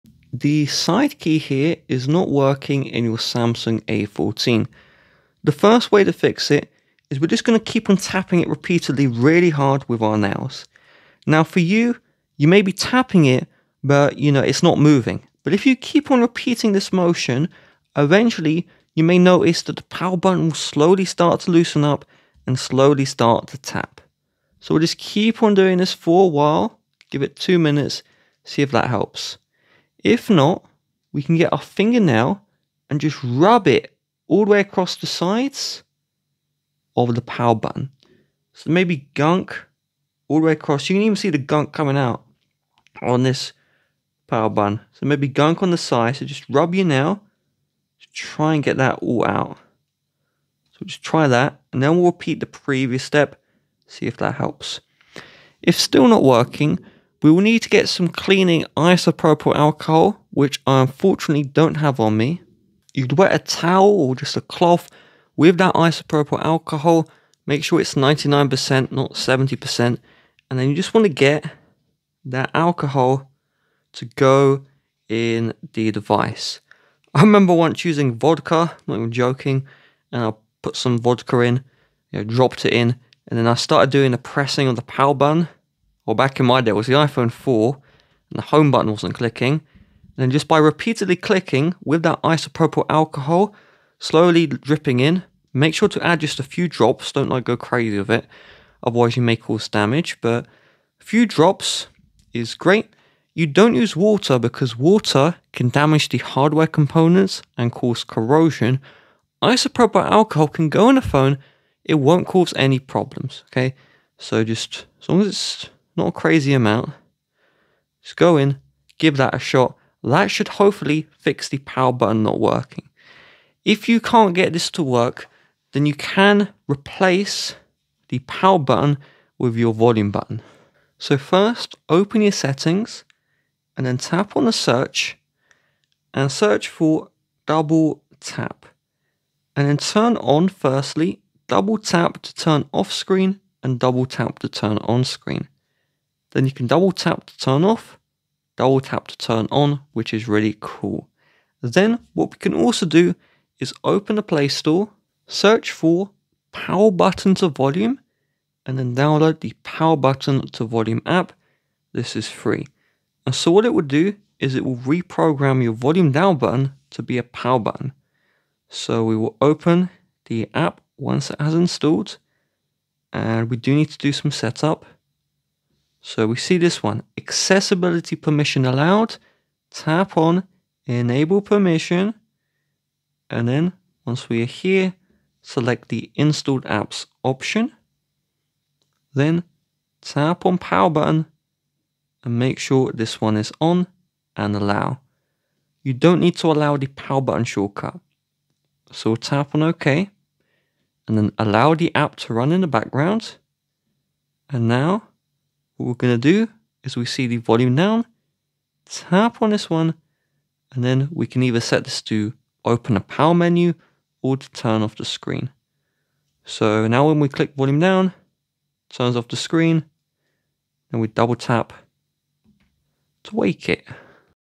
Samsung Galaxy A14 Side/Power/Lock Button Not Working? 4 Fixes!
Welcome to our troubleshooting guide for fixing the side/power/lock button not working on your Samsung Galaxy A14! In this video, we’ll show you four effective fixes to resolve issues with your device’s buttons. Whether it's a hardware problem, a software glitch, or a settings issue, we’ve got you covered. Follow along to get your Galaxy A14 buttons working properly again.

This video applies to both Samsung Galaxy A14 and Samsung Galaxy A14 5G